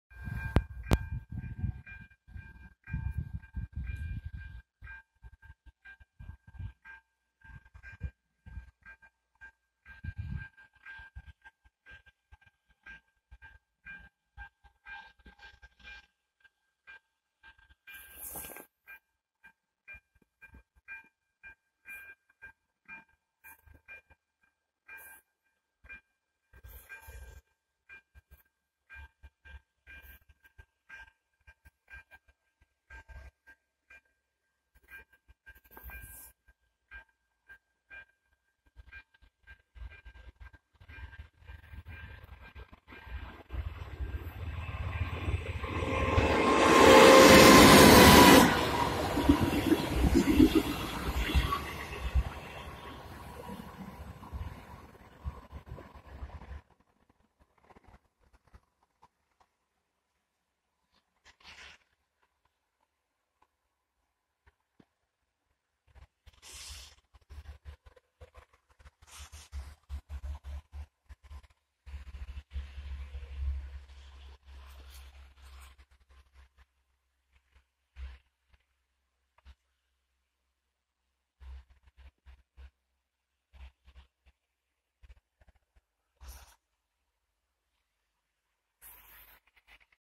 Zasněžená Veselá Kráva 714 018, Rakev 843 024,Bftn791 009 na zpožděném R 1327 u stanice Krnov-Cvilín
Od 20.března do 24.dubna 2020 a od 8.března 2021 do 19.dubna 2021 jezdily rychlíky linky R27 bez vozů Btn753. Od prosince 2021 budou rychlíky R27 přečíslovány (např. R 1327 bude R 847). Dne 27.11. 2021 se v ranních hodinách na R 1332 ve stanici Opava východ porouchala Rakev, opět 843 014 a do Olomouce odjela s 50 minutovým zpožděním a s Veselou Krávou 714 018, kterou jsem párkrát točil (na R 1333 v noci a na R 1331). Zpožděním ale není konec! Na R 1327 se s 843 024 vracela zpátky do Opavy a z Olomouce odjela s 45 minutovým zpožděním. V Miloticích předjela Osobák a v Krnově měla 55 minut zpoždění. Mě se jí podařilo natočit na klasickém místě, kde jsem ji točil i na R 1331.